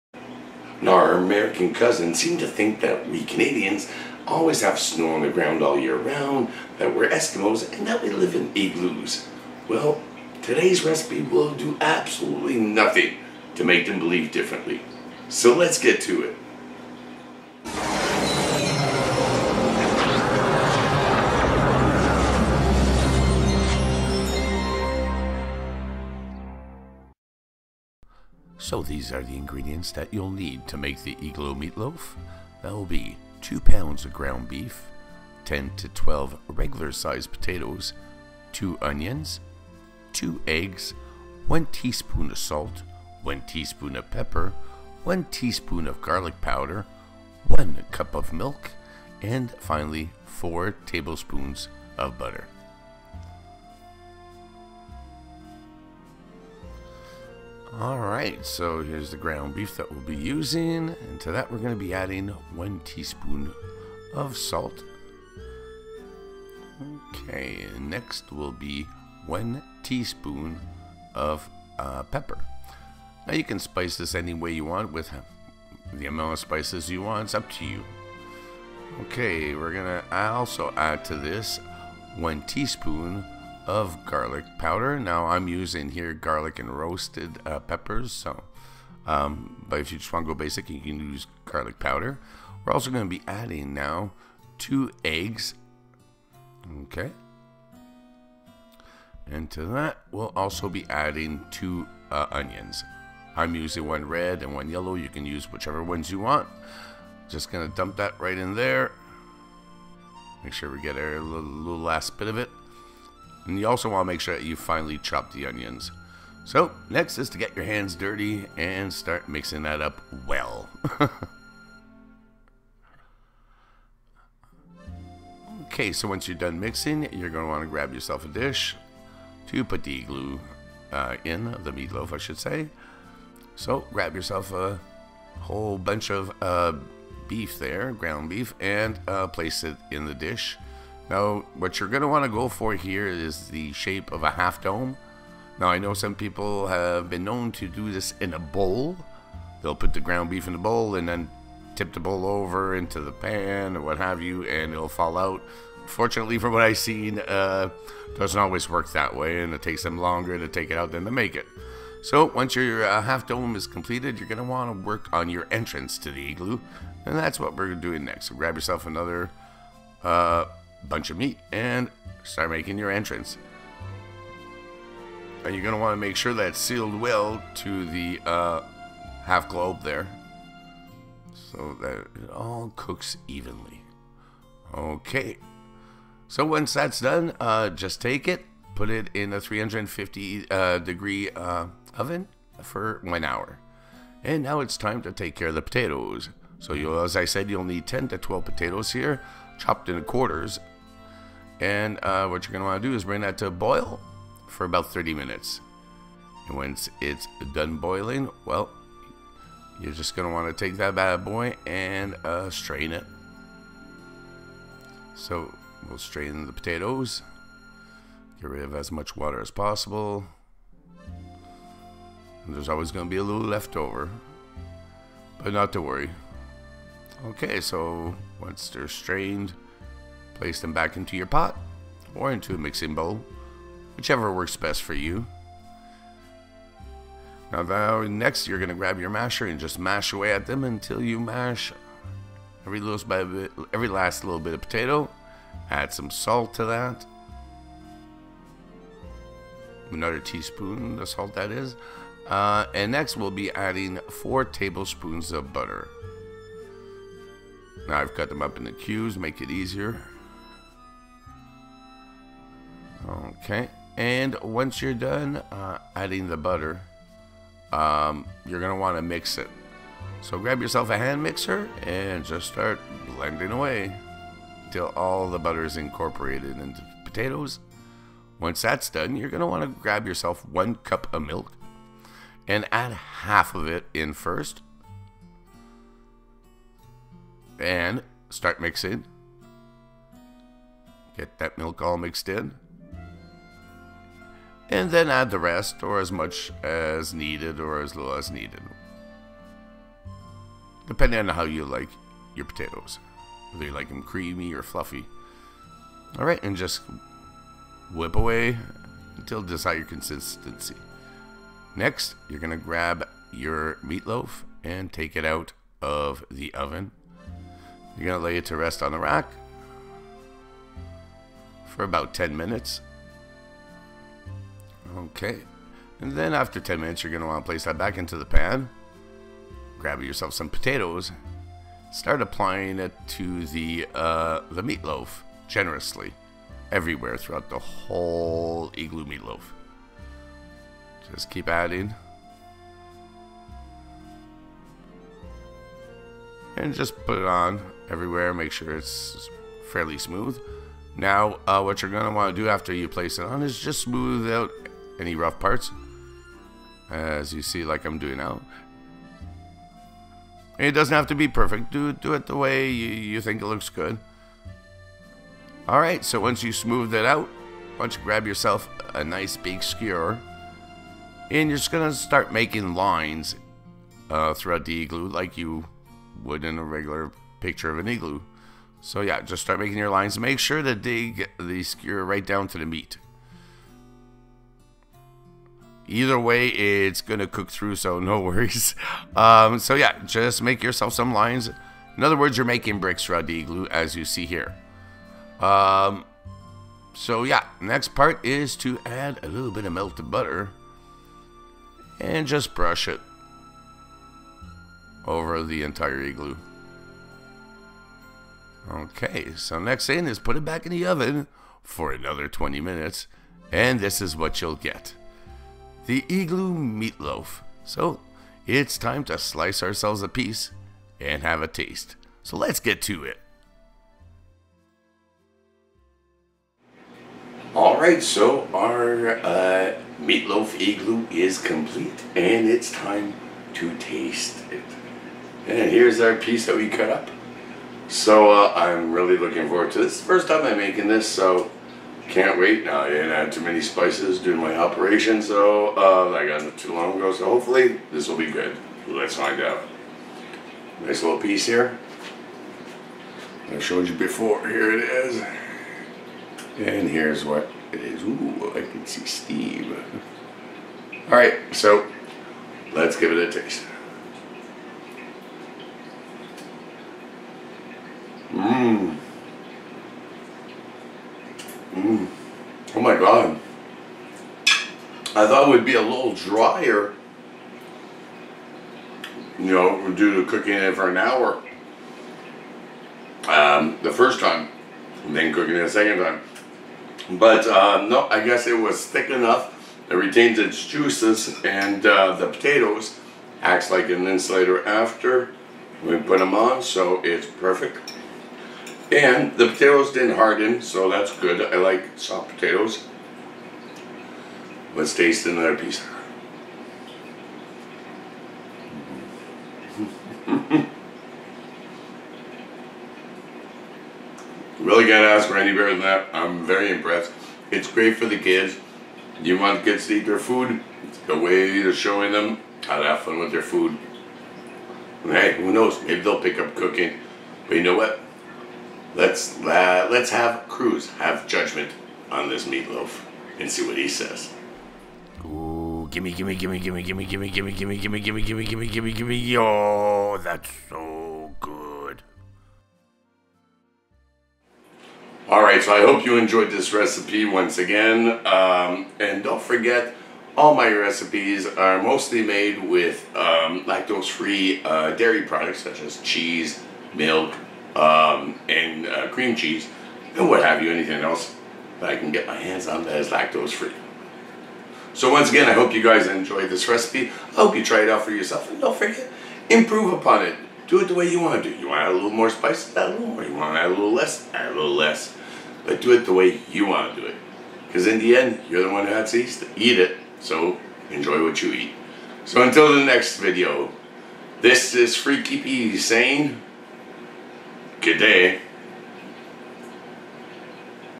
Igloo Meatloaf / Freaky Fun Foods
Igloo Meatloaf Recipe:

Ingredients:

2 pounds of Ground Beef
10-12 regular sized potatoes
2 Onions (chopped)
2 Eggs
1 teaspoon of Salt
1 teaspoon of Pepper
1 teaspoon of Garlic powder
1 cup of Milk
4 tablespoons of Butter

Cooking Instructions:

Place ground beef in a large bowl
Add 1 teaspoon of salt
Add 1 teaspon of pepper
Add 1 teaspoon of garlic powder
Add 2 eggs
Add 2 chopped onions
Mix all these ingredients together well

Get a large oven casserole dish, and place about two thirds of the ground beef mixture in the casserole in the shape of a half dome.
Take remainder of ground beef and shape it like a tunnel to create the entrance
 to the igloo. Make sure the meat from the entrance is connected to the half globe.
Place in a preheated 350 degrees Farenheit oven, and cook for one hour.

Boil 10-12 potatoes (cut into quarters) for about 30 minutes on medium high heat.
Strain potatoes.
Put potatoes back in pot or in a big bowl, and begin mashing potatoes with a  potato masher.
Add 4 tablespoons of butter
Mix potatoes with a hand mixer
Slowly add milk to the potatoes and mix with the mixer.

Take meatloaf out of the oven, and place on rack to cool for about 10-15 minutes.
After 10-15 minutes, place meatloaf back in casserole dish.
Begin to apply mash pottaoes generously to cover the entire meatloaf.
Smooth out the mash potatoes, and using a skewer, make lines in the shapes of bricks into the mash potatoes. Try to make the lines as deep as possible.
Brush some melted butter over mash potatoes and put in a preheated 350 degrees Farenheit oven, and cook for 20 minutes.

Serve & Enjoy